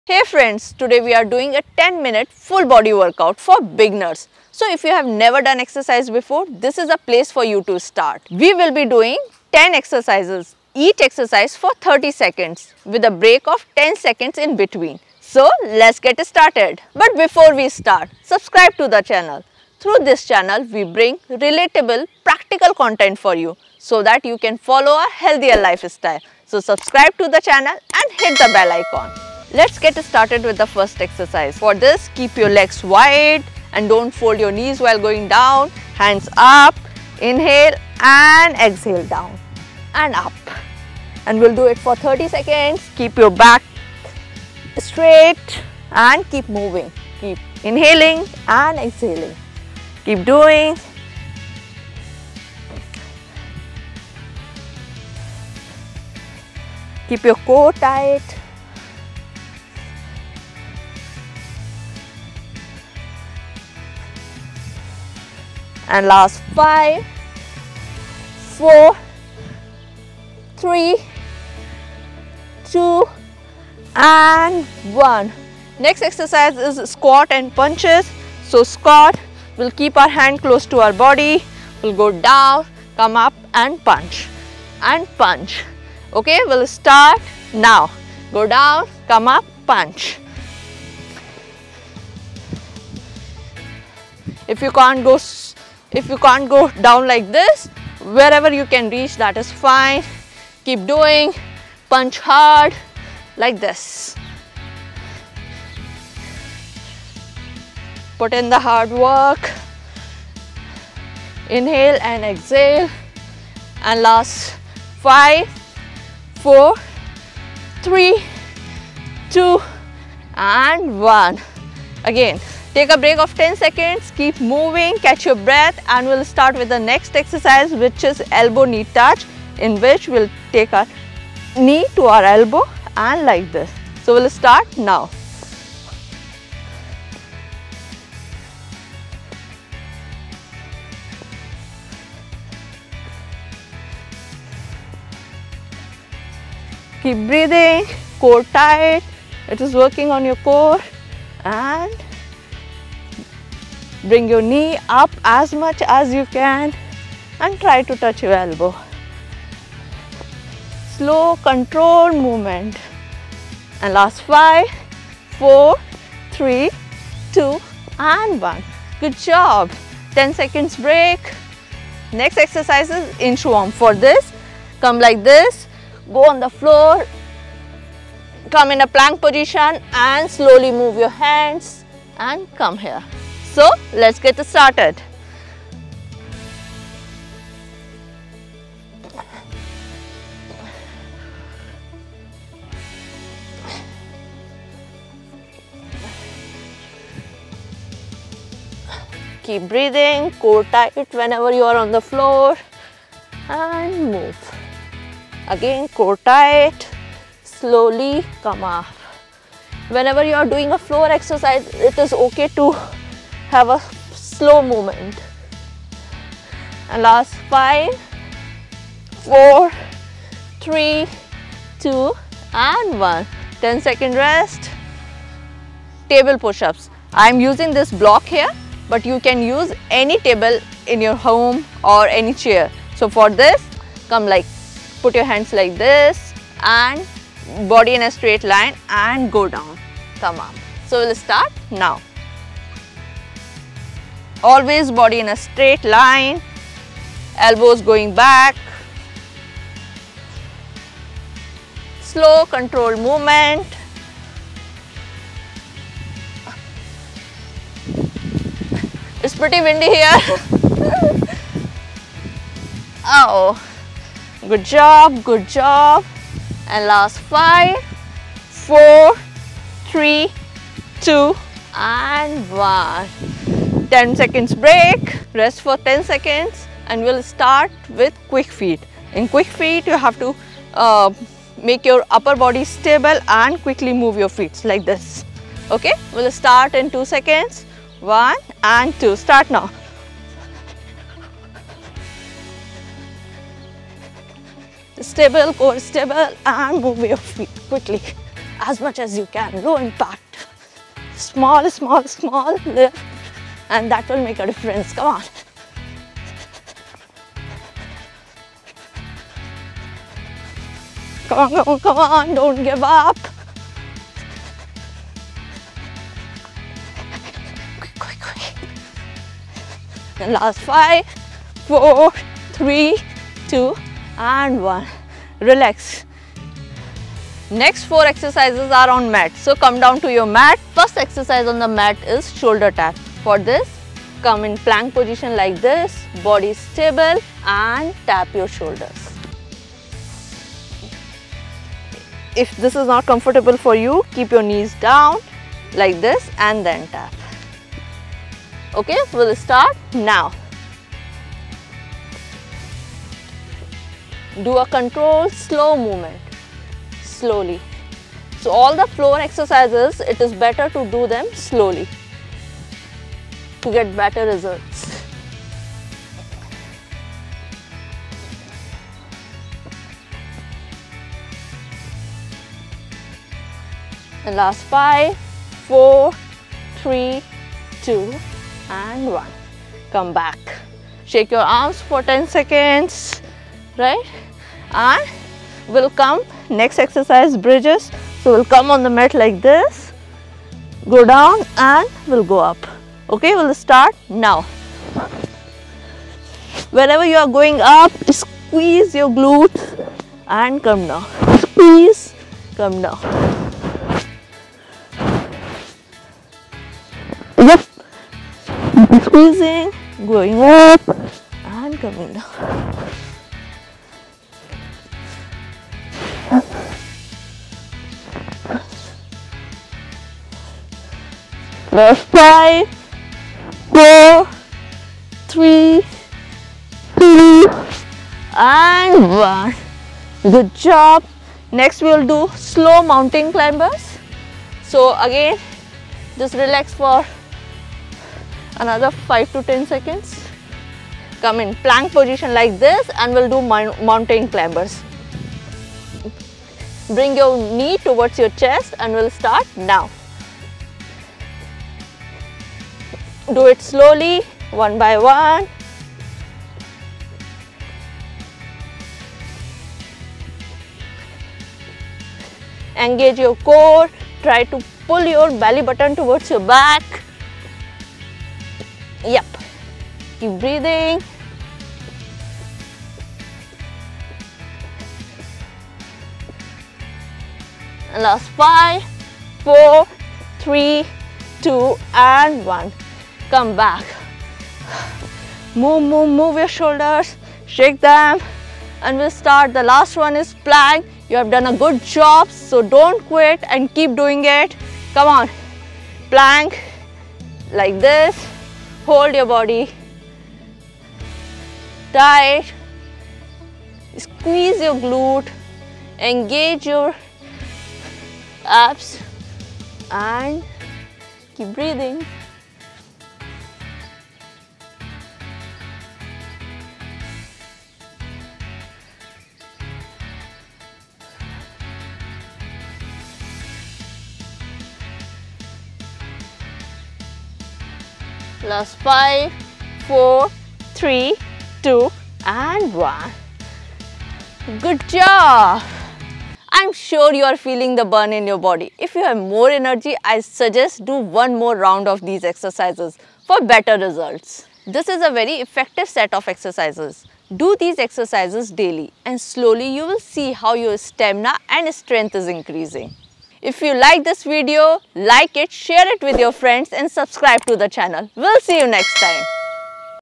10-Minute HOME WORKOUT | No Equipment Full Body Exercise
Here is a quick full body no equipment home workout for beginners inspired by GunjanShouts to lose weight and build strength. If you are looking for a workout sequence to include in your daily routine, start with this. Perform 2 sets of this sequence regularly and you will soon see an increase in your strength and stamina.